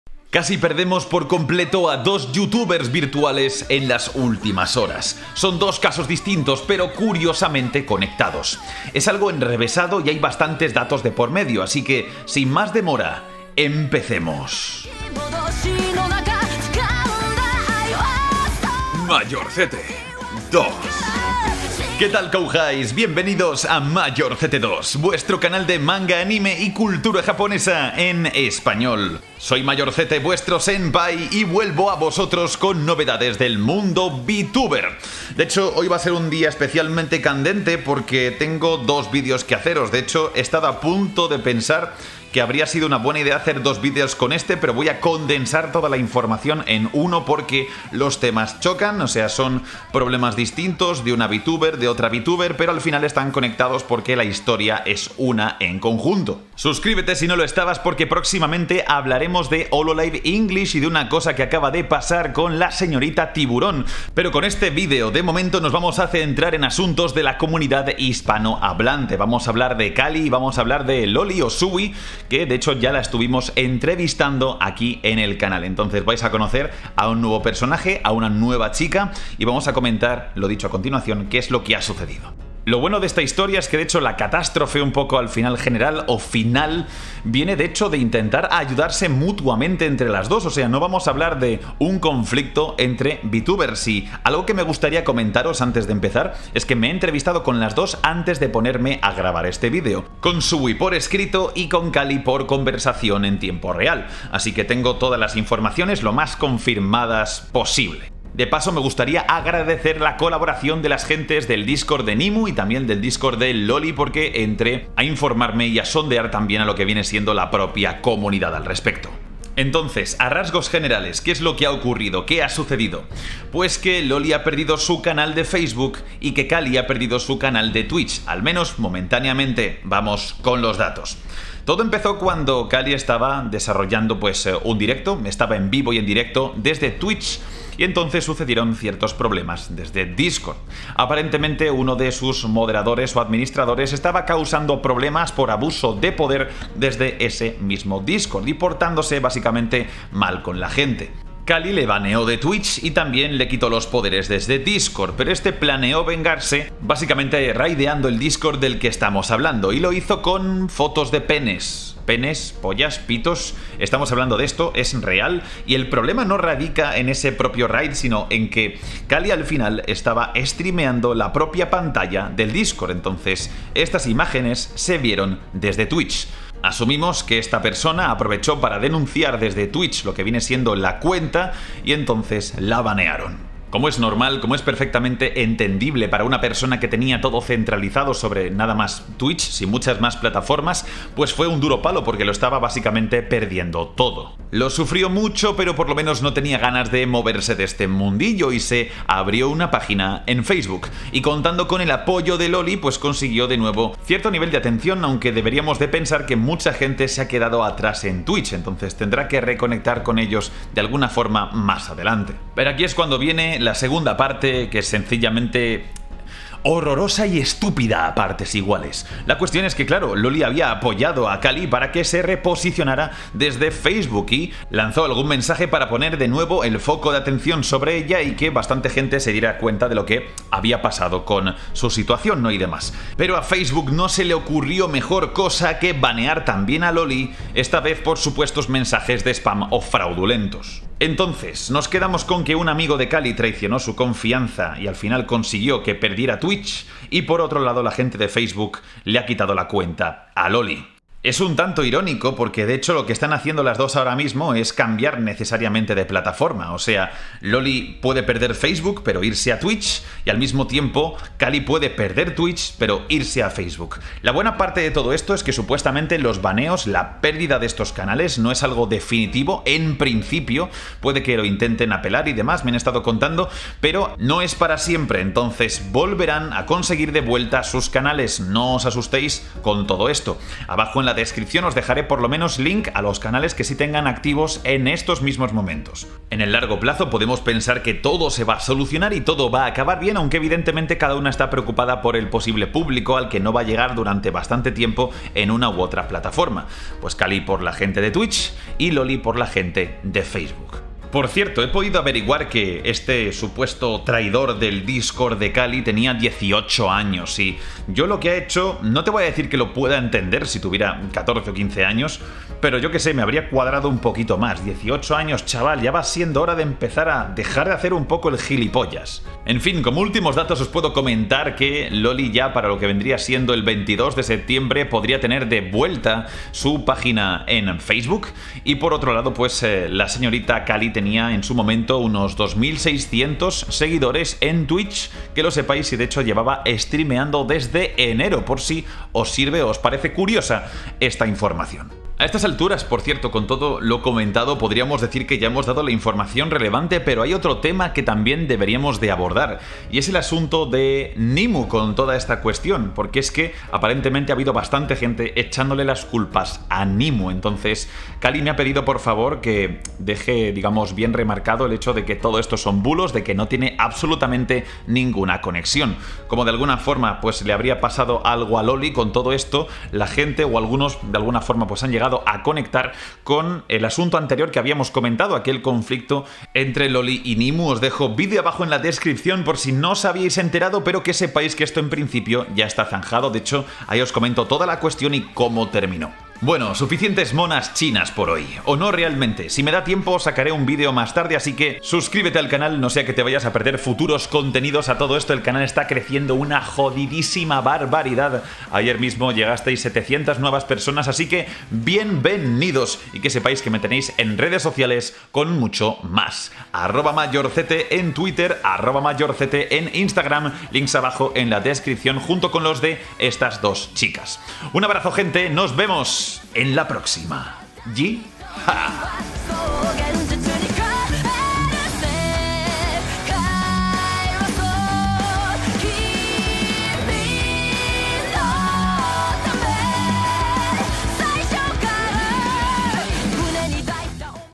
2 Vtubers latinas PIERDEN SUS CANALES de forma INJUSTA 【KALI】【SUWIE】
KALI y SUWIE pierden sus canales de forma INJUSTA. NOTICIAS de VTUBERS en ESPAÑOL